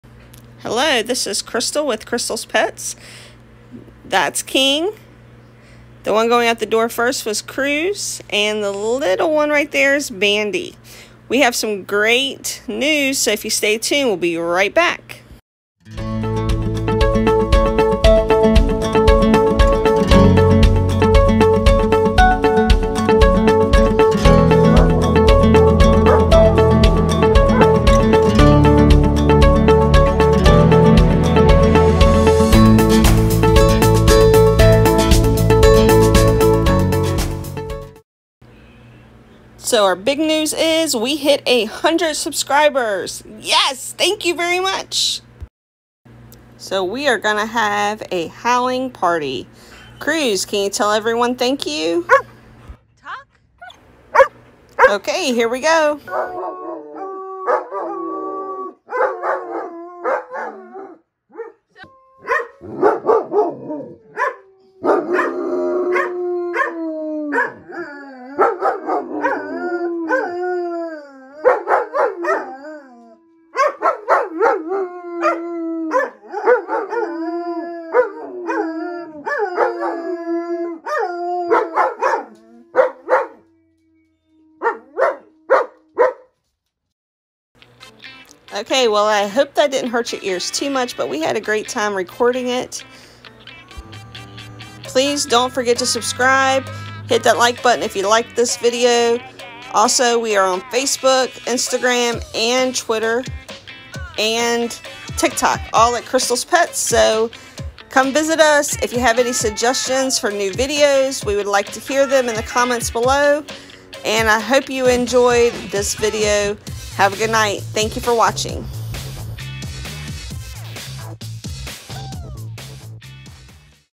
Dutch Shepherd & German Shepherd Howling Party
dutchshepherd germanshepherd howling Big News! Please come and watch Cruz are Dutch Shepherd and King are German Shepherd Celebrate in a Howling party 🎉